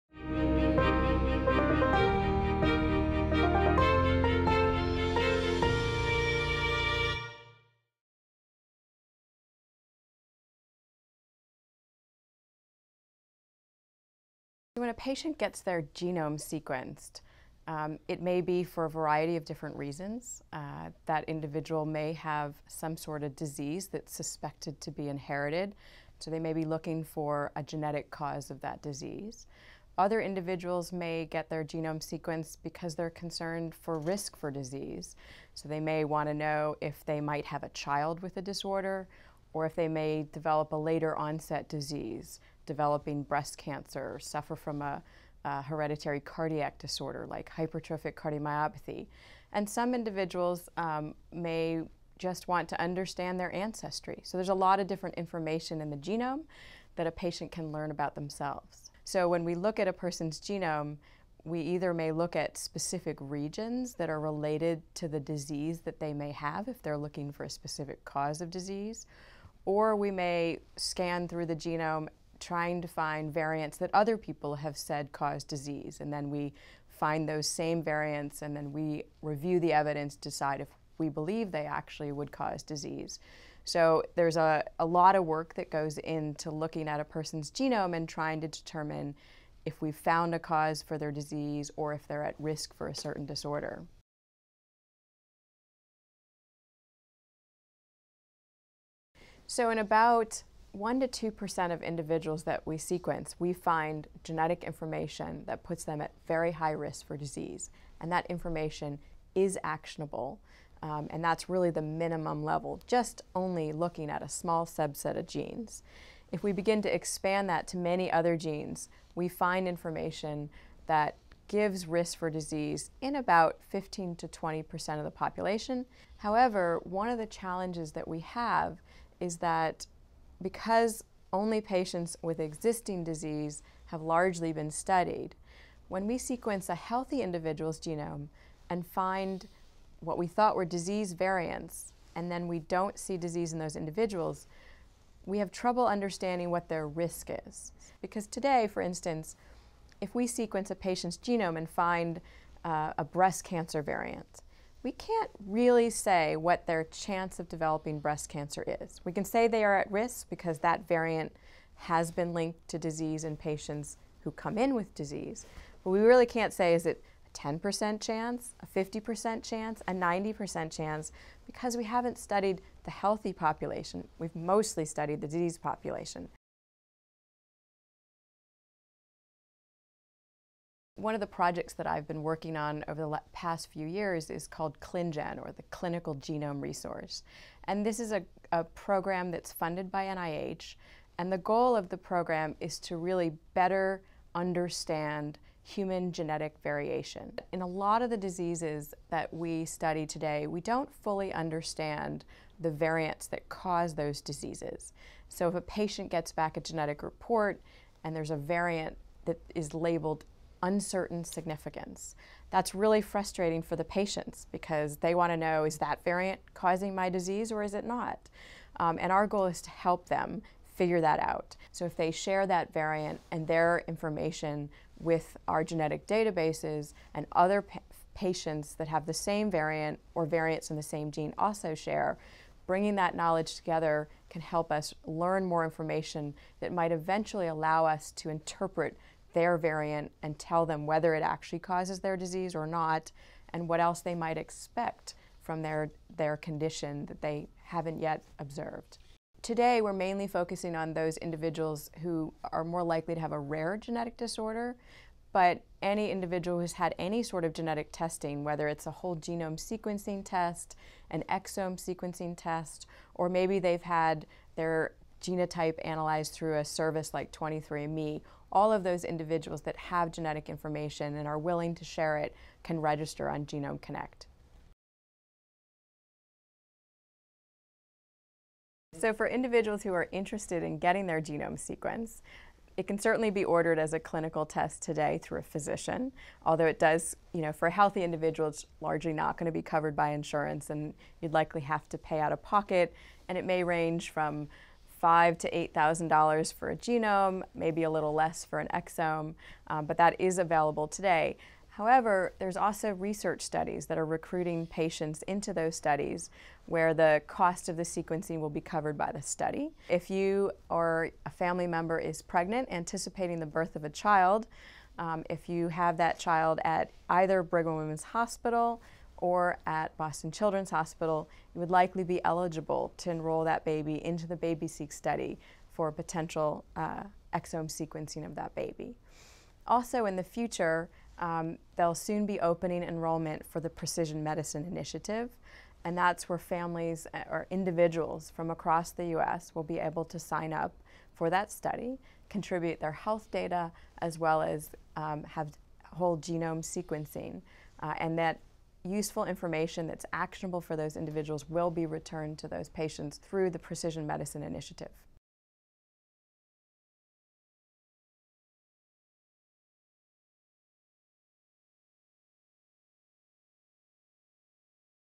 Understanding Genomic Sequencing Video - Brigham and Women's Hospital
Heidi Rehm, PhD, Associate Molecular Geneticist at Brigham and Women’s Hospital and Director, Partners HealthCare Laboratory for Molecular Medicine, describes the different types of genomic sequencing tests that are available to patients and what the results reveal about current and future health.

Patients may have their genome sequenced for a variety of different reasons. They may be looking for the genetic cause of an existing disease, they may be concerned about the risk of developing a disease later in life,  they may want to find out if they are a genetic carrier for a disease that may be passed on to a child, or  they may simply want to better understand their ancestry.

When analyzing a genome, geneticists may look at specific regions that are related to a disease or they may scan through the genome trying to find variants that other people have said cause disease. In about one to two percent of individuals who have their genomes sequenced, geneticists find information that puts patients at very high risk for disease and that information is actionable.

In approximately 15 to 20 percent of the population, geneticists find variations that suggest a risk for developing disease; however, it can be difficult to quantify the likelihood that healthy individuals may develop disease because because researchers have mostly studied the genomes of patients who have already developed a disease.

The Clinical Genome Resource (ClinGen) is a research program funded by the National Institutes of Health whose goal is to help understand the significance of genetic variations by consolidating genetic test results from individuals into a single database.  Any individual who has undergone genetic testing can share their genetic test results through the ClinGen program.